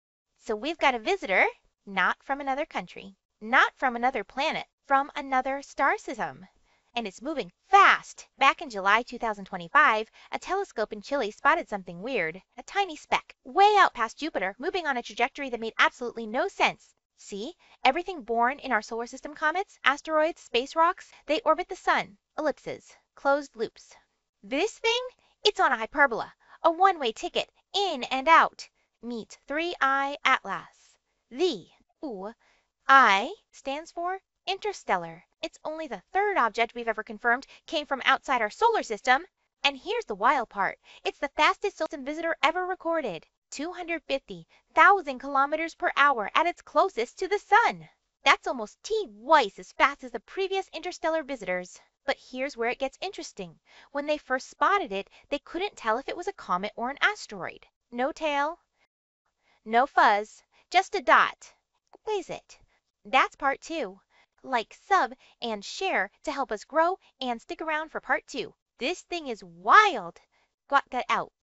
We Have a Visitor From Another Star | Izzy (1/4)
Something is falling through our solar system. And it's not from here.

In July 2025, a telescope in Chile spotted an object 410 million miles out. Moving wrong. Too fast. Trajectory too steep. The math said it came from interstellar space — another star system entirely.

They named it 3I/ATLAS. The third confirmed interstellar object humanity has ever seen.

But here's the thing: it's the fastest one yet. 250,000 kilometers per hour at its closest approach to the Sun. That's not orbiting. That's passing through.

We've had two visitors before — 'Oumuamua in 2017 and Borisov in 2019. Both gone now. Both left more questions than answers.

This one's different. We caught it early. We're watching it. And what we're finding is weird.

This is Part 1 of a 4-part series on 3I/ATLAS — the interstellar comet that's rewriting what we know about objects from beyond our solar system.

💬 Comment: If you could send ONE message to another star system, what would it say?